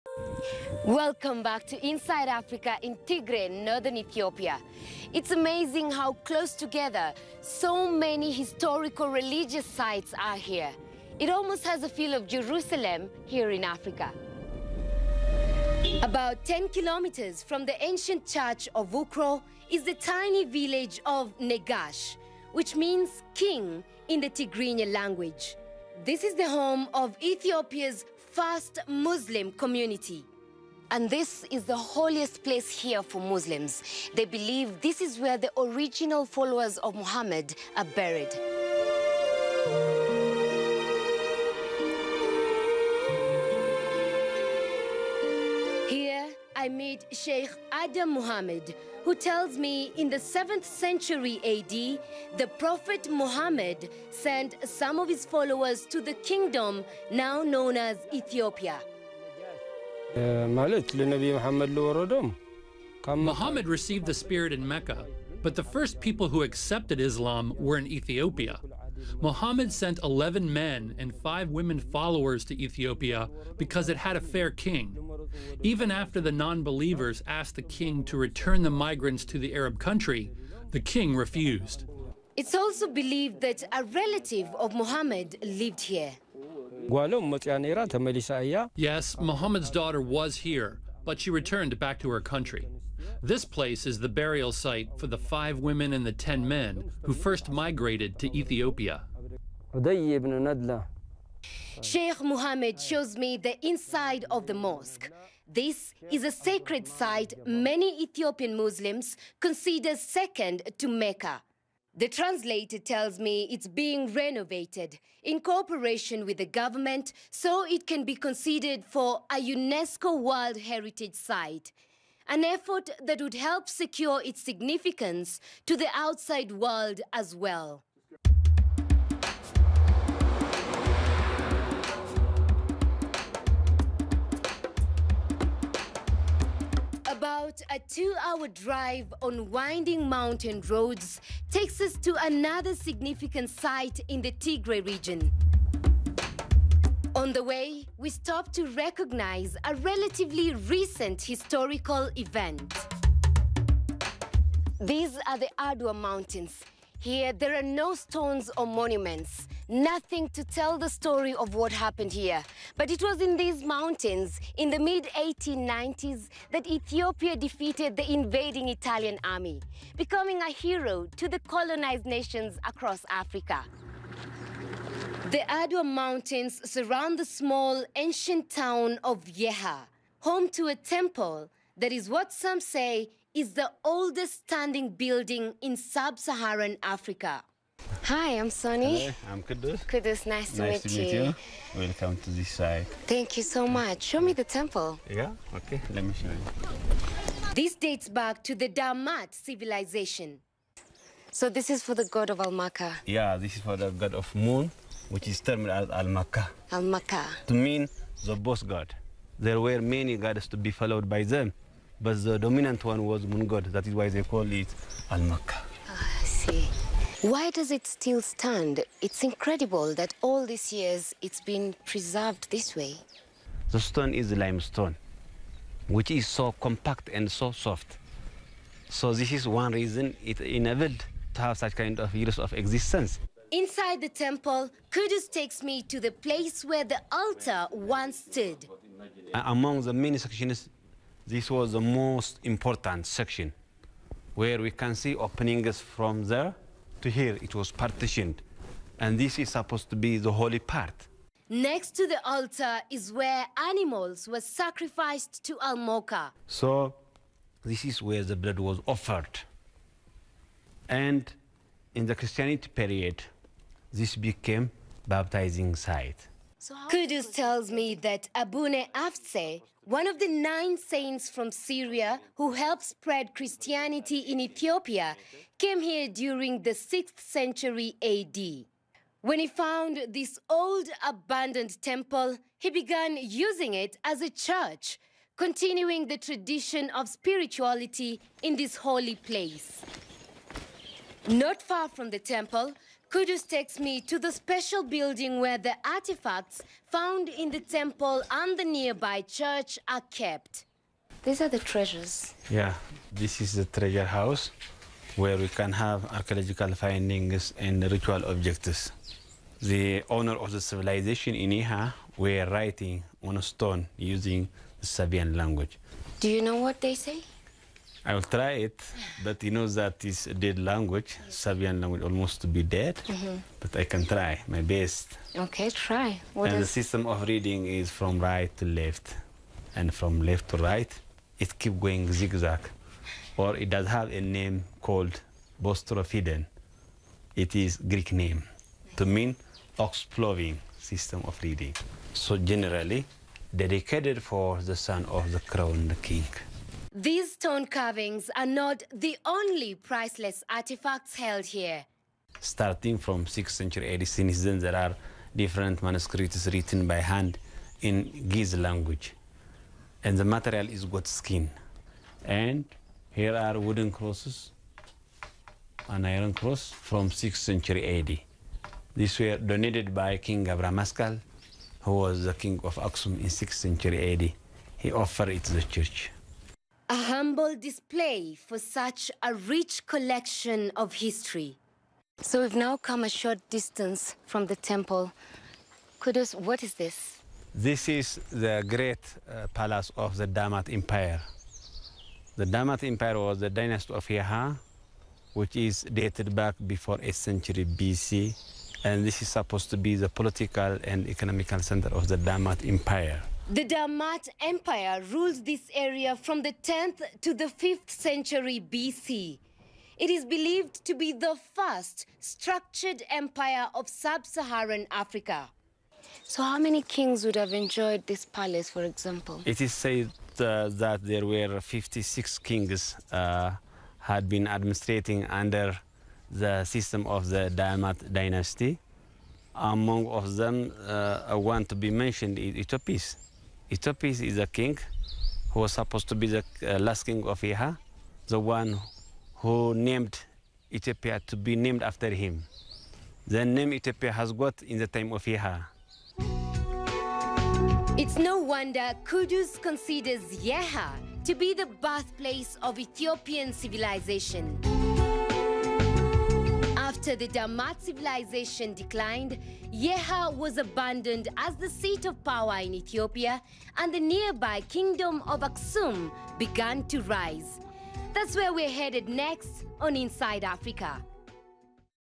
The Cradle of Ethiopian Islam - CNN
Tigray, Ethiopia (CNN)Ethiopia is often overlooked as a top destination for spiritual pilgrimage. This is an unfortunate oversight.

The country is not just the cradle of civilization, it has played a significant role in the formation of many of the world's top religions. It is not only the location of the biblical kingdom of Sheba, it is currently believed by some to house the Ark of the Covenant. Scroll through the gallery above for a list of the country's top religious sites.